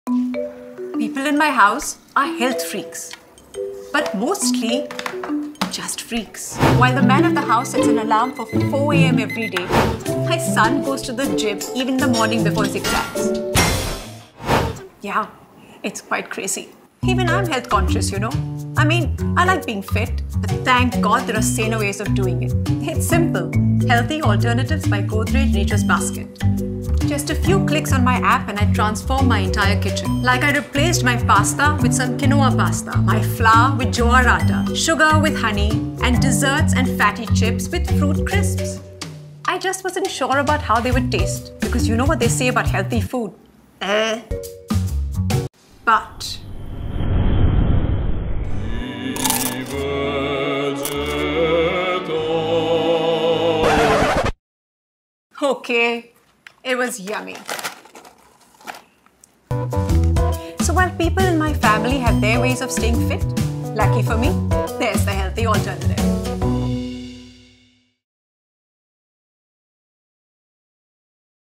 Twinkle Khanna | Healthy Alternatives Range | Godrej Nature's Basket
Twinkle Khanna divulges simple ways to stay healthy with Godrej Nature's Basket's Healthy Alternatives range.

Client: Godrej Nature's Basket
Client Team: Kavita Palekar, Karthik Ranganathan, Shaana Levy-Bahl
Agency: Schbang
Agency Team: Harshil Karia, Disha Daswani, Rajan Mahalingam, Aashay Shah, Akshay Gurnani
Production House: Schbang Motion Pictures
Director: Sayan Mukherjee
Producer: Souraj Bhattacharyya
Co-Producer: Harshil Karia
DoP: Malay Prakash
Screenplay Writer: Sayan Mukherjee, Disha Daswani, Harshil Karia
Unit Production Manager: Durgesh Dadhich
Art Director: Vidhi Gurnani
Costume Stylist: Priyanka Kapadia
Hair & Makeup: Mickey Contractor
1st AD: Shaunak Vyas
Asst. DoP: Vishal Singh chauhan
Gaffer: Vishal Singh Chauhan
Storyboard Artist: Nitin R Malekar
Chief Accountant: Suryakant Bondre
Editor: Adith Anande
Colorist: Adolf D'souza
Sound Mix & Master: Adith Anande